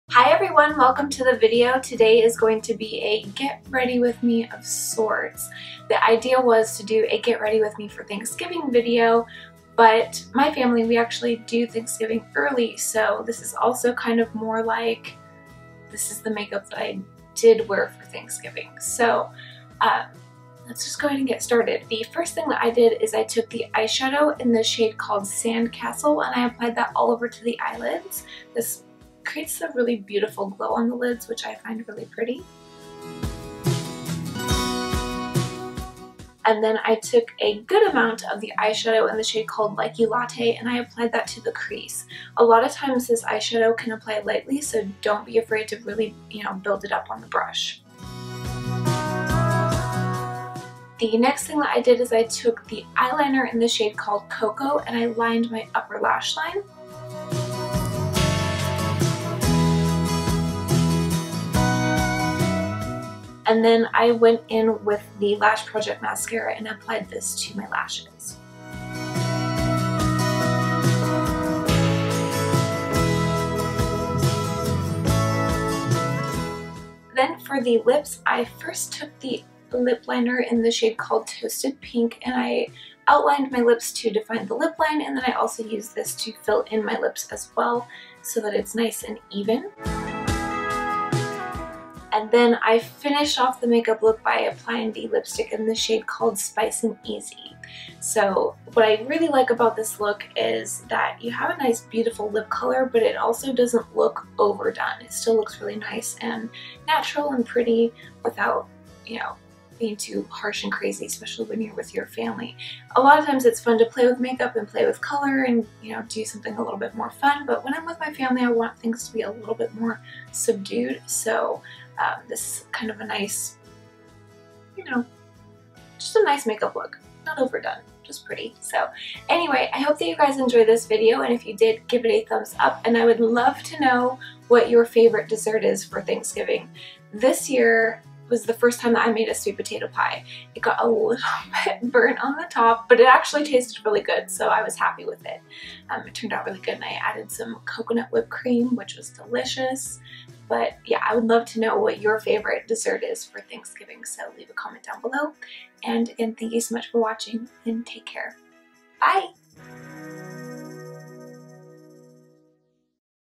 Thanksgiving Makeup Tutorial Ft. Spice 'N Easy Lipstick
Ashley shares with you the makeup she's wearing for Thanksgiving2019 using the new Spice 'N Easy lipstick and Toasted Pink lip liner.

LIPSTICKS AND LINERS LAUNCH ON 11/29

PRODUCTS MENTIONED:
Spice 'N Easy lipstick
Toasted Pink lip liner
Sand Castle eyeshadow
Like U Latte eyeshadow
Cocoa eyeliner
The Lash Project mascara